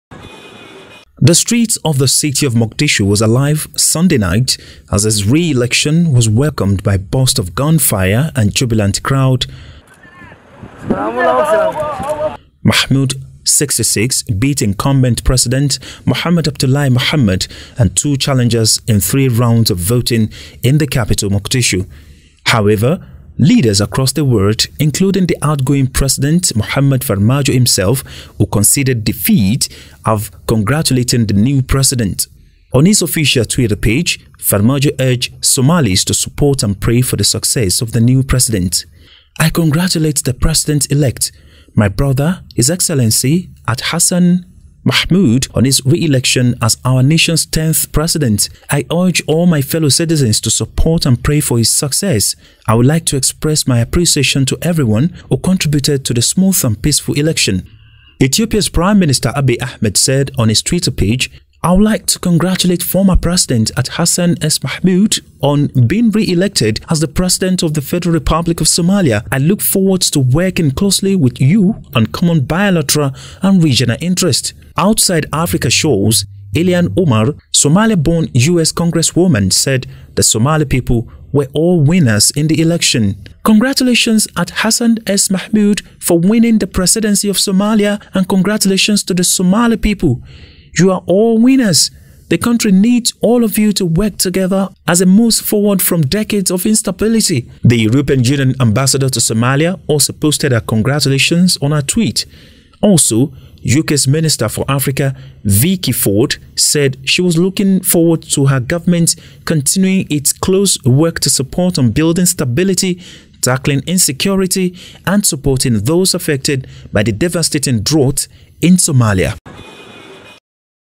Congratulatory messages pour in as Somalia elects new president
Leaders around the world including the voted out ex Somalia president Muhamad Farmaajo himself have been tweeting congratulatory messages to Somalia's new President-elect Hassan Sheikh Mohamud for his hard won victory in Sunday's presidential election.